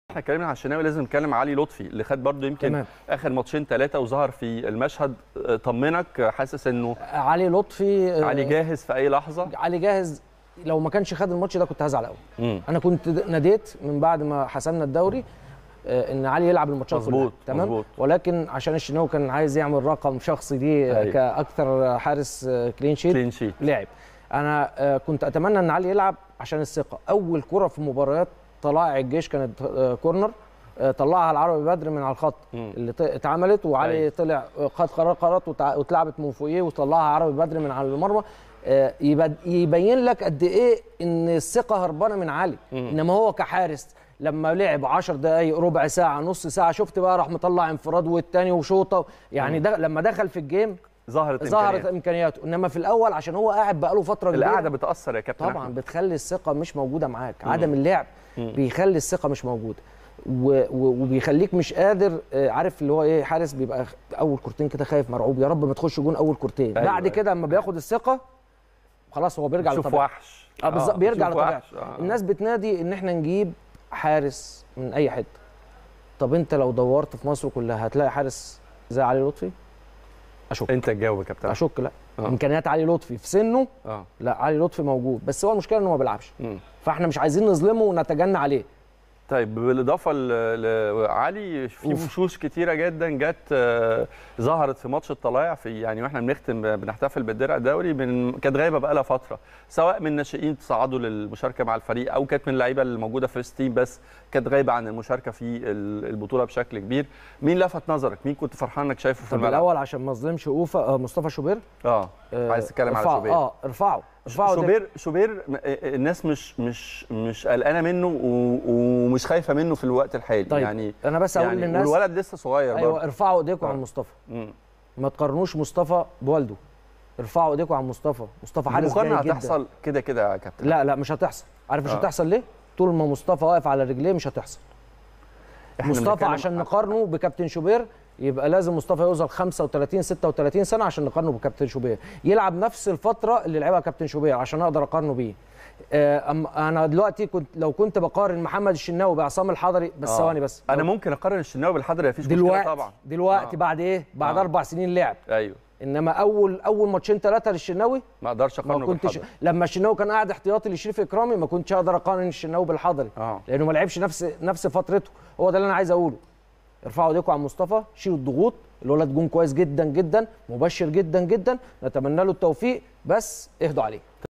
فوزي: "علي لطفي" حاس فذ.. وارفعوا إيديكم عن "مصطفى شوبير" ولا تتدخلوه في مقارنة مع أبوه | السادسة
ضيف الفقرة
أحمد فوزي ـ حارس مرمى النادي الأهلي السابق